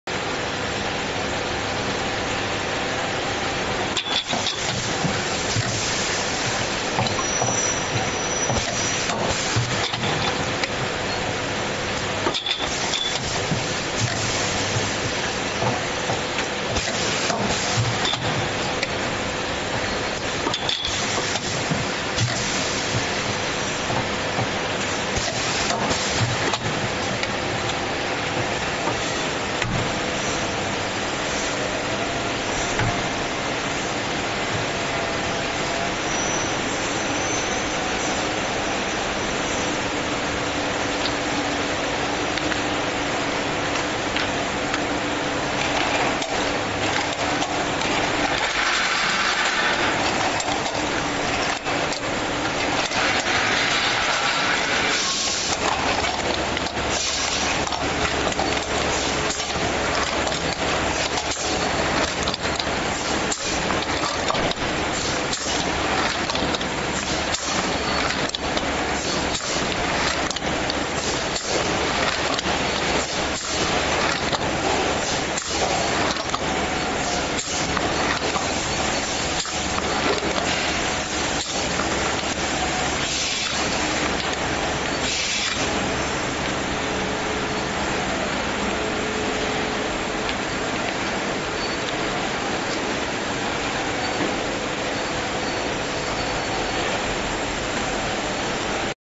CHILI SAUCE GLASS BOTTLE PISTON FILLER,WITH TWIST OFF VACCUM CAPPER,
CHANGZHOU QINCHENG PACKING MACHINERY CO;LTD

OVAL SHAPE BOTTLE SHAMPOO PISTON FILLING,LOTION CAPPING MACHINE, التلقائي بالكامل ملء آلة الشامبو
Major characteristics:
1. The filling machine is suitable for syrup, oral liquid, EC, suspending agent, honey, soy sauce, washing liquid, cream and other products can be widely used in pharmaceutical, chemical, food, such as lubricating oil in various industries.
2. servo motor control system and imported material seal filling measurement overall adjustment, ensure the measurement precision and stability.
3.Meet with GMP standard requirements, each pipeline connected by fast loading, convenient disassembly and cleaning, and material contact parts and exposed parts are made of high quality stainless steel material,
4. this machine adopts PLC control, touch screen operation, has the characteristics of easy, convenient adjustment and high stability.
II. Main Technical Parameter:
1. Capacity: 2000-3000 bottles/hr. customized
2. Applicable Range: Φ（40-100）*（80－230）mm Round Bottle
(40－100) * (40－100) * (80－230) mm Flat / Square Bottle
3. Filling Range：50ML-1000ML
4.Filling head: 1-20 nozzles customized
5. Precision：±0.5%
6. Air Pressure：0.6～0.8 MPa
7. Power Source：～380V、50Hz customized
8. Power：2.5KW
9. External Dimension：2000×1150×2300mm
10. Weight：About 600Kg